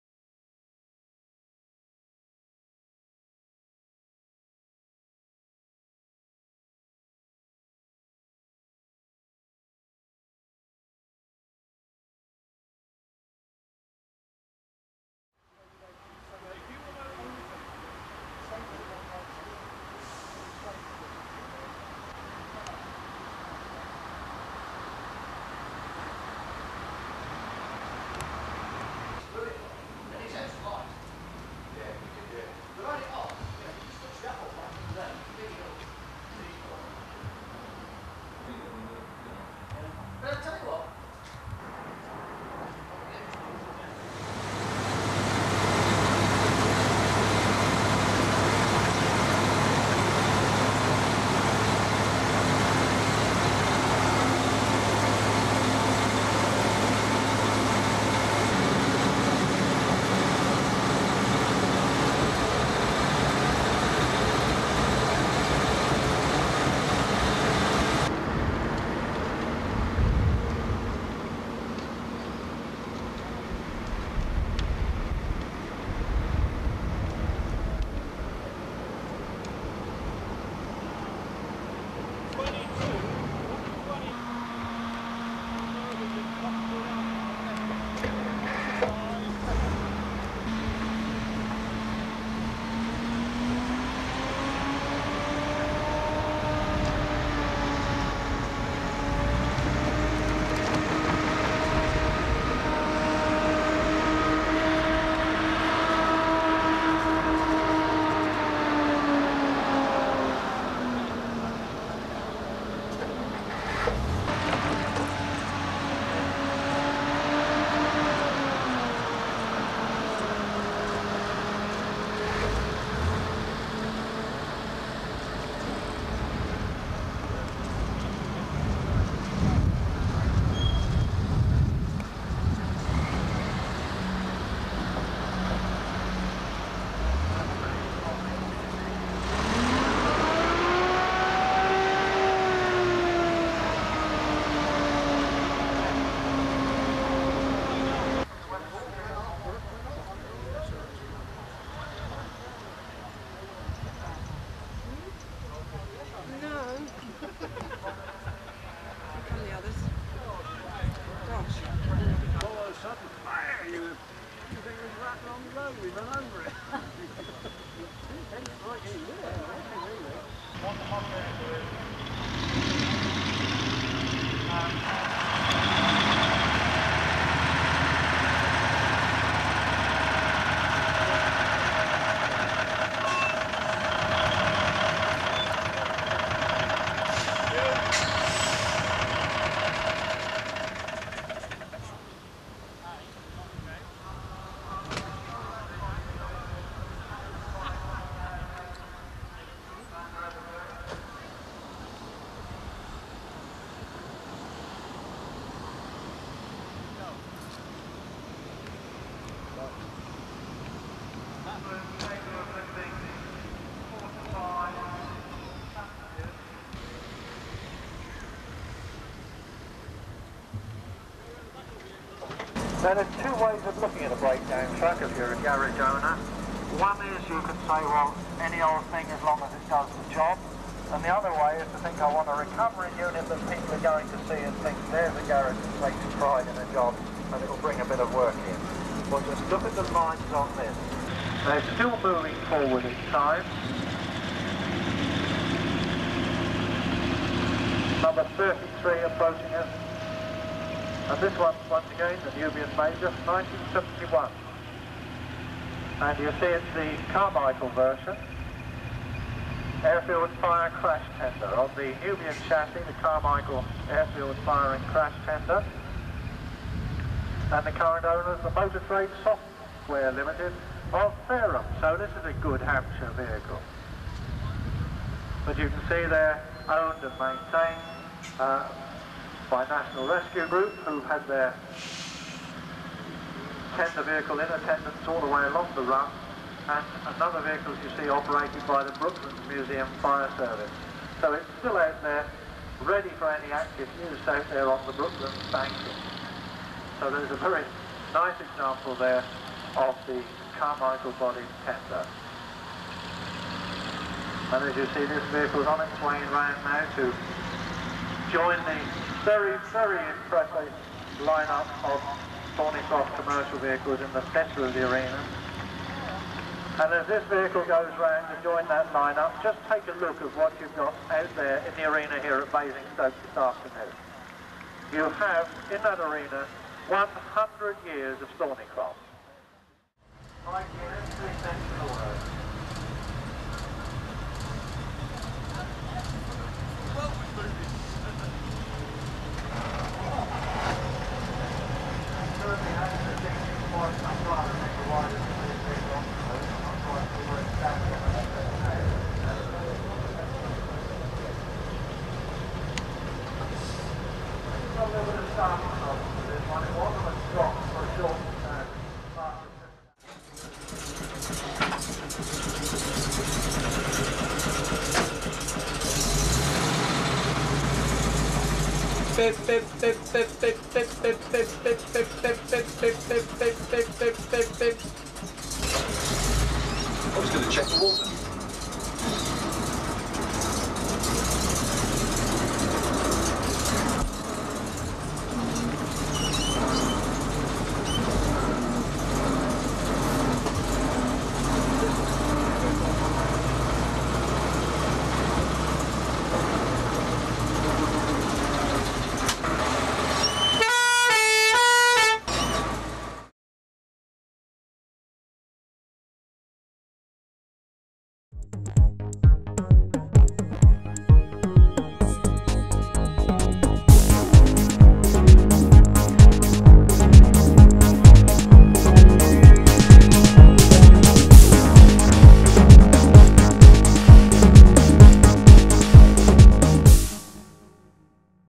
Sun Nubian Crash Tender in 1996 Thornycroft Centenary Road Run
In 1996 myself and a few friends took part in the Thornycroft Centenary Road Run, which went from Chiswick to Basingstoke and then on to Winchester. Regrettable the film is very poor quality, as the tape has degraded over the years. However I am sure it will be of interest to Thornycroft fans and others who like old heavy vehicles. We entered both the Thornycrofts we owned at the time and of special interest to me was the Sun Nubian, because it had been built in the same yard at Cambridge Road Kingston where my Company National Rescue was later formed. There is more of this sort of thing